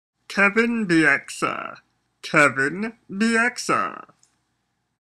How to Pronounce Kevin Bieksa Vancouver Canucks NHL Hockey Player
Learn the proper way to say and pronounce the word Kevin Bieksa hockey player for the NHL Vancouver Canucks in English. Hockey Fight Shootout Goal Hit. Pronounce and say English words correctly. English and American pronunciation.